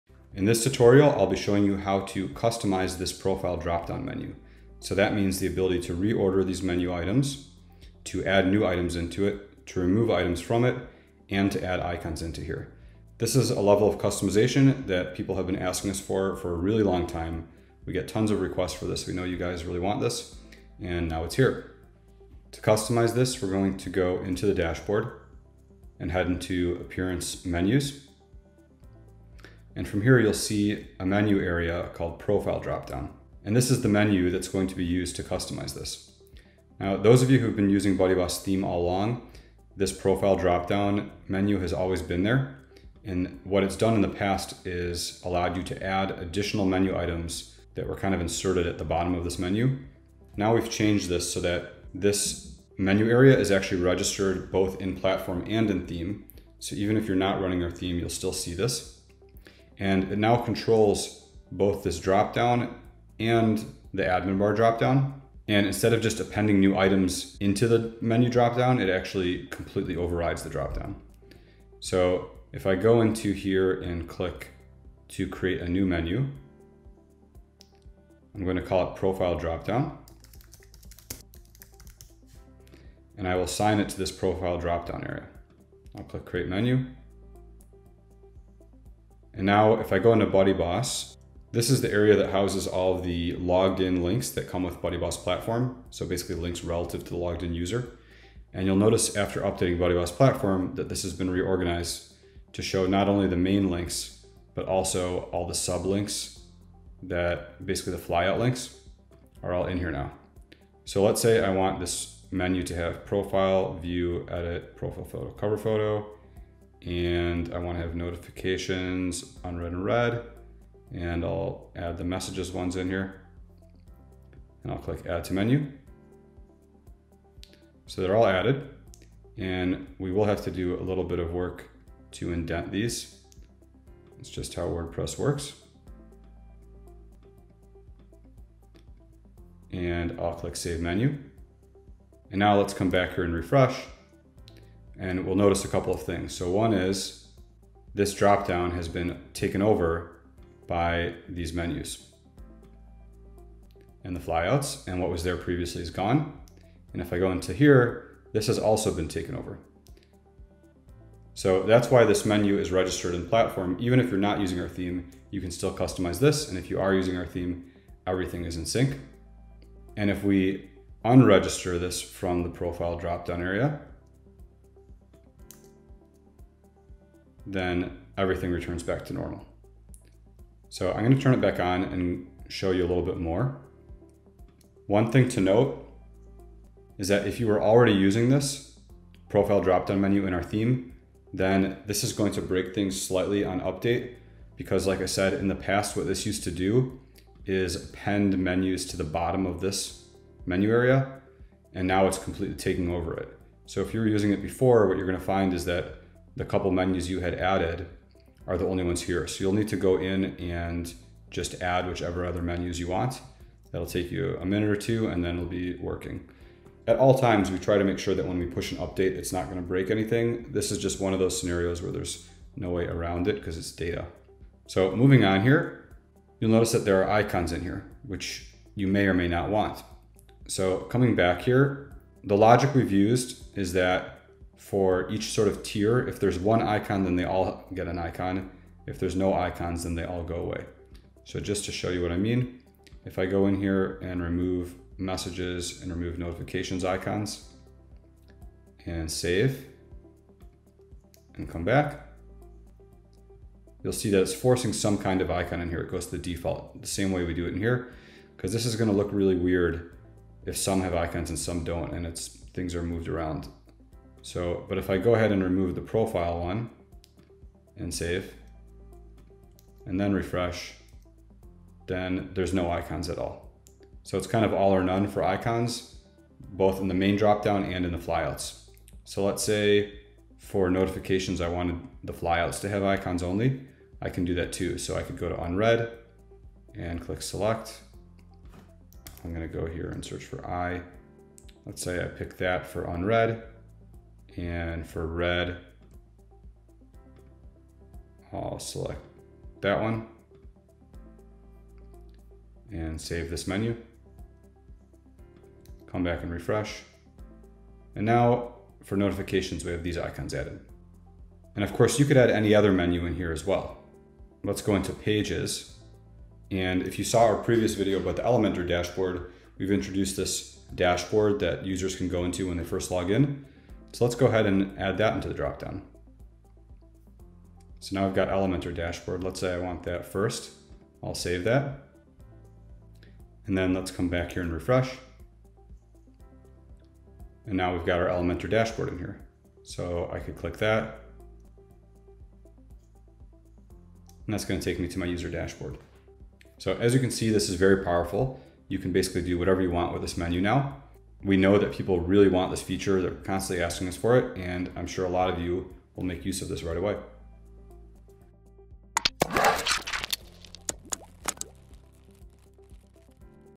How to Customize the Profile Navigation Dropdown | BuddyBoss Platform Tutorials
In this tutorial, you will learn how to completely customize the Profile Navigation dropdown. You can now add menus, remove menus, re-order menus, and add custom icons to any menu in the dropdown. This also overrides the profile menus in the WordPress admin bar.